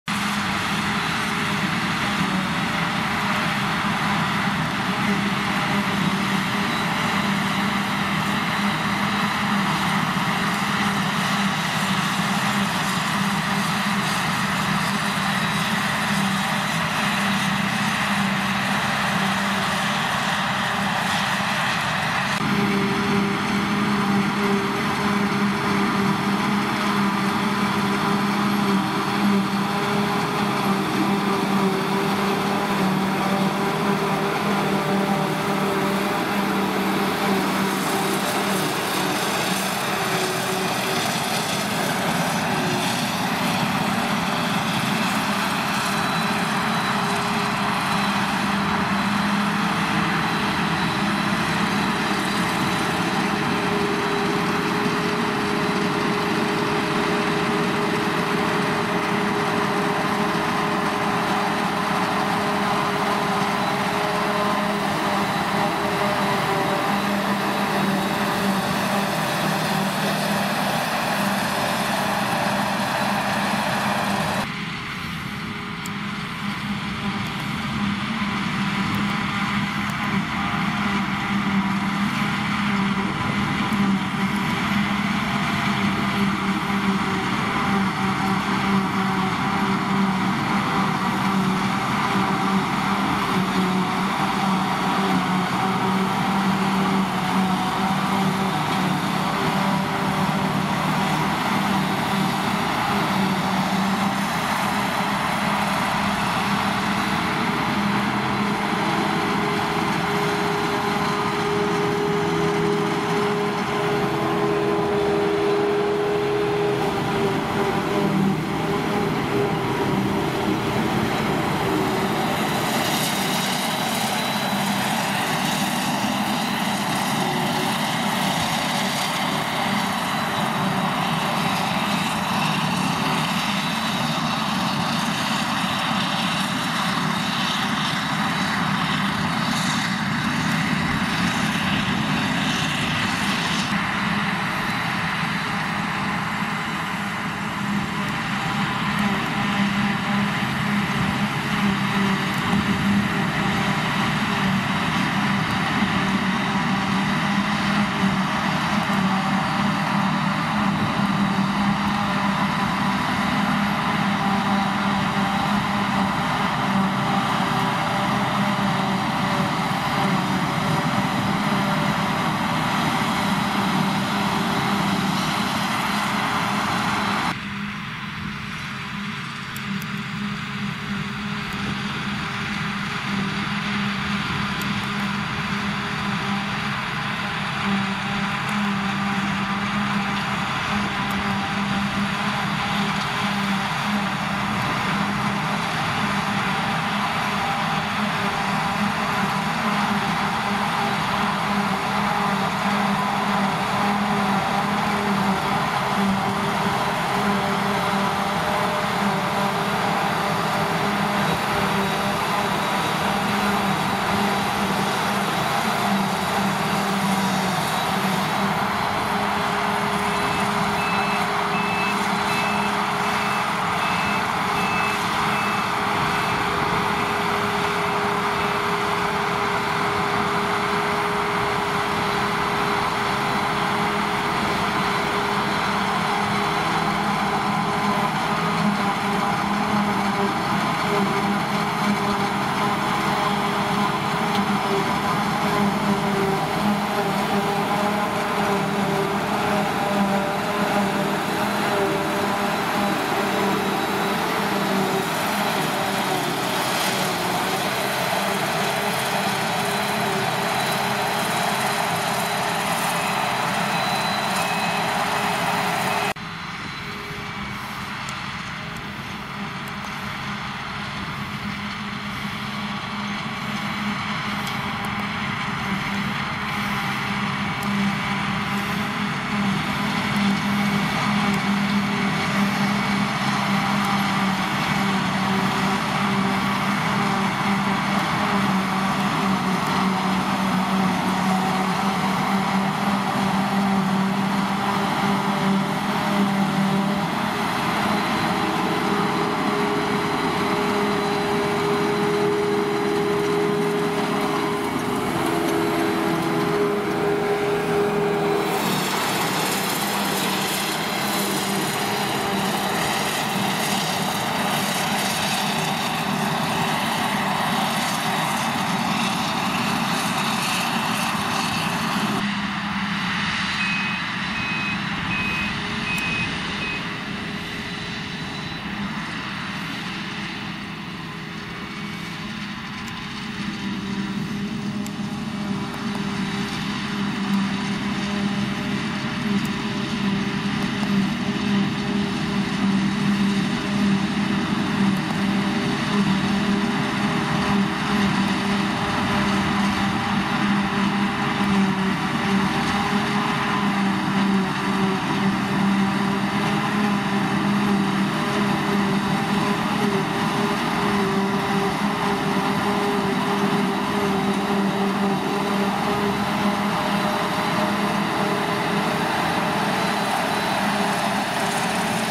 Silage 2022 Murphy's of Watergrasshill Part 1
Silage 2022 - The team from Murphy's of Watergrasshill at first cut silage near Glenville County Cork with a Claas Jaguar 970 Forage Harvester and a fleet of John Deere, Claas & New Holland Tractors with Broughan Engineering Trailers on carting duties. A word of thanks to all involved